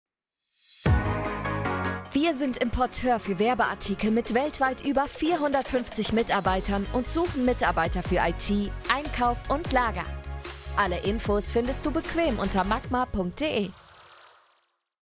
Macma Kinospot
Erleben Sie die Magie von Macma, dem Werbeartikelhersteller aus Nürnberg, in unserem packenden Kinospot. Tauchen Sie ein in eine Welt kreativer Möglichkeiten und hochwertiger Lösungen, die Ihre Marke erstrahlen lassen. Willkommen bei Macma – wo Werbeartikel zu unvergesslichen Botschaftern werden.